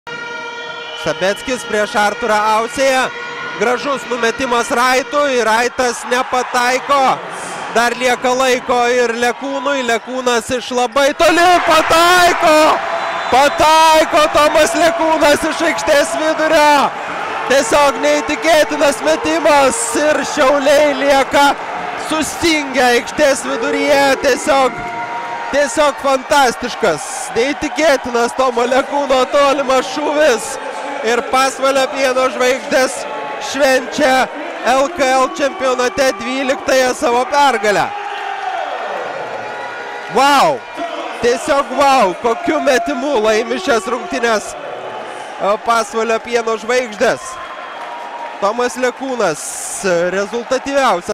Game-winner buzzer-beater half-court shot by Tomas Lekūnas
Tomas Lekunas drained a half-court buzzer-beater to hand Pieno Zvaigzdes a dramatic 84-81 victory in Lithuanian Basketball League (LKL).